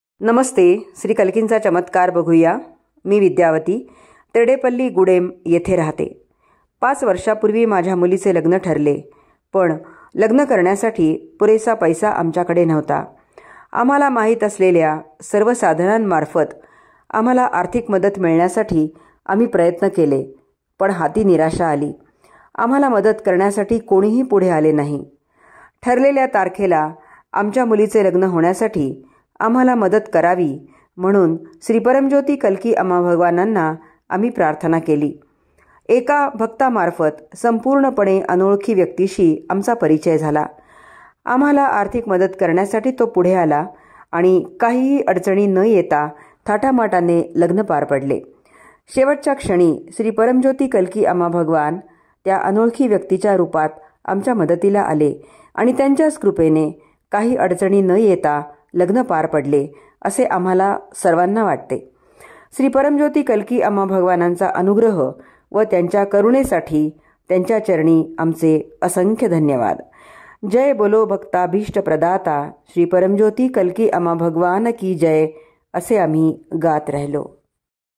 30/11 श्री कल्कि अम्मा भगवानांच्या कृपेने माझ्या मुलीचे लग्न पार पडले.
पाच वर्षापूर्वी माझ्या मुलीचे लग्न ठरले पण लग्न करण्यासाठी आमच्याकडे पुरेसा पैसा नव्हता. ठरलेल्या तारखेला आमच्या मुलीचे लग्न होण्यासाठी आम्हाला मदत करावी म्हणून श्री परमज्योति कल्कि अम्मा भगवानांना आम्ही प्रार्थना केली. श्री परमज्योति कल्कि अम्मा भगवानांचा अनुग्रह व त्यांच्या करूणेसाठी त्यांच्या चरणी आमचे असंख्य धन्यवाद.